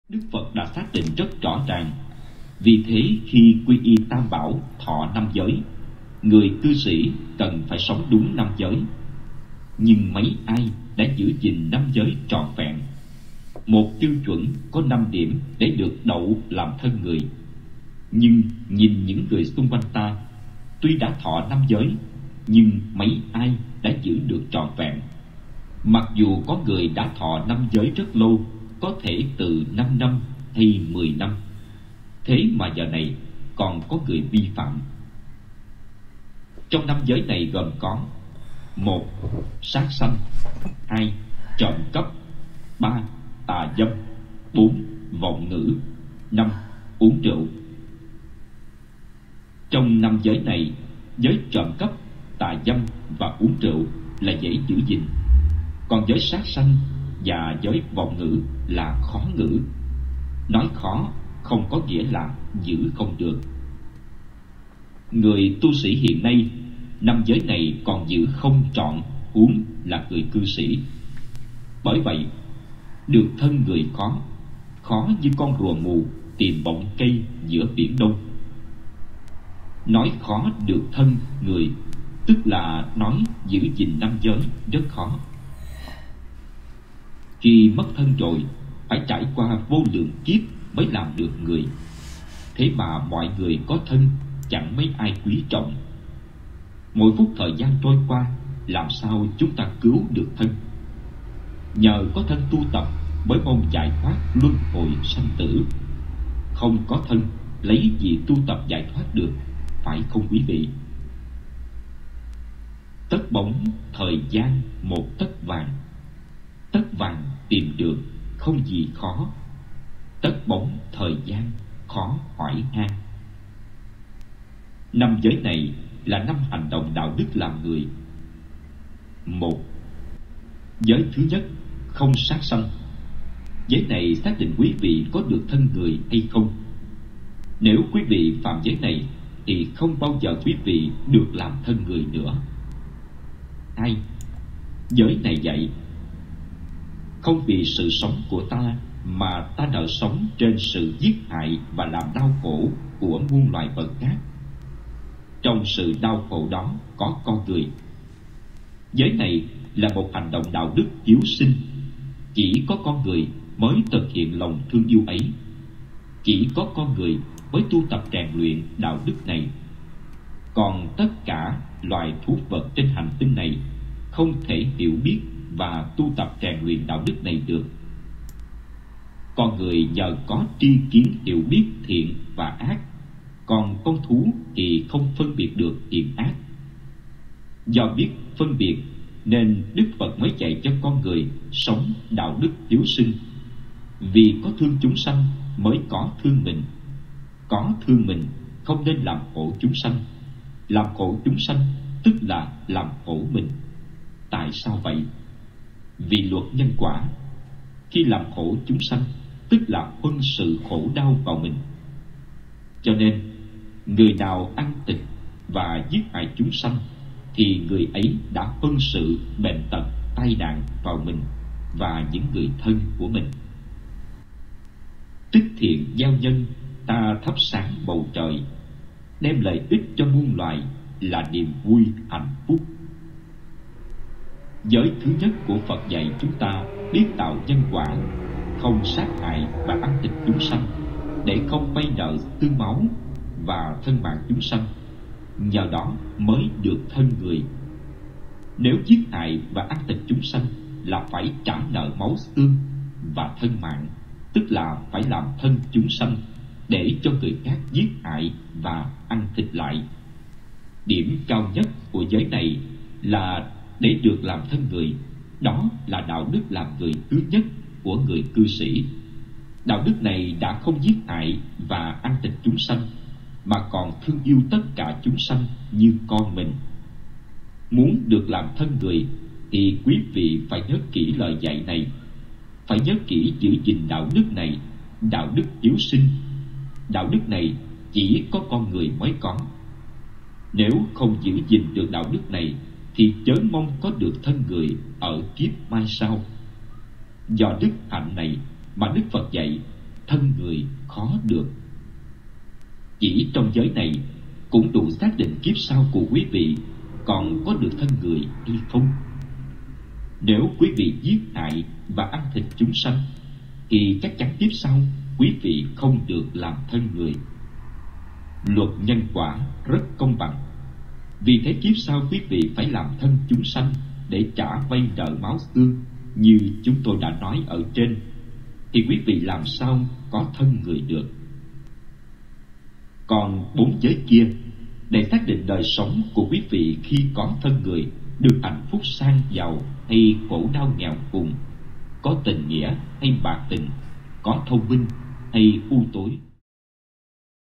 Sát sinh và ăn thịt chúng sinh không được tái sinh làm người! Alahan Thông Lạc giảng.
Theo luật nhân quả, sát sinh và ăn thịt chúng sanh vay nợ xương máu thì phải tái sinh làm chúng sanh để bị giết và bị ăn thịt.